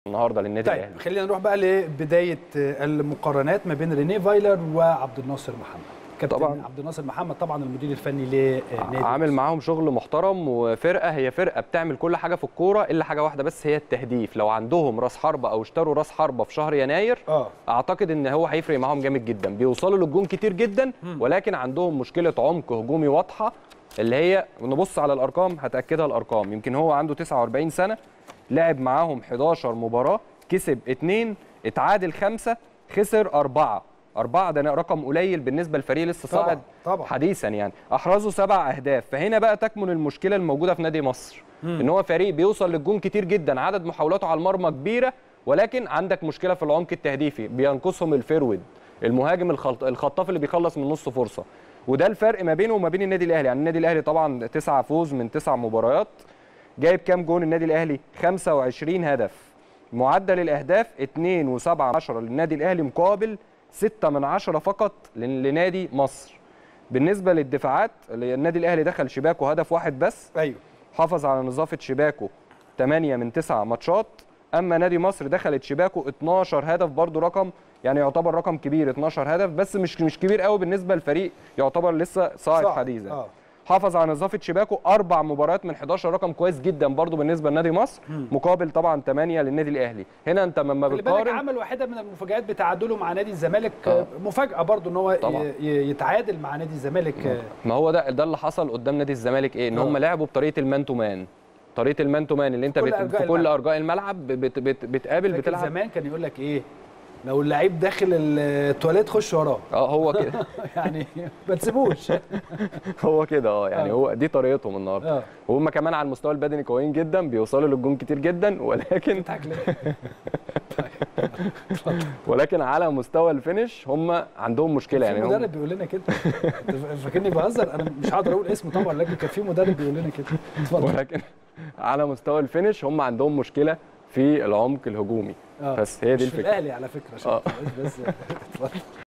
أحمد ثابت يستعرض مقارنة بين رنيه فايلر وعبد الناصر مدرب نادي مصر
التردد: 12341 أفقي.
القمر: نايل سات.
معامل الخطأ: 5/6.
معدل الترميز: 27500